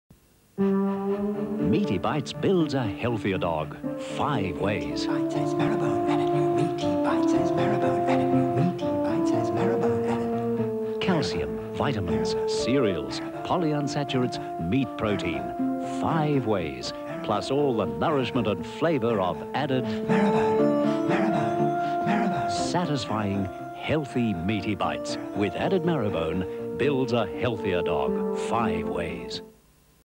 Meaty-Bites Dog Food - 1985 Australian TV Commercial
Recorded from TCN-9 Sydney during a broadcast of the film "Annie" at 7:30pm on Friday October 11, 1985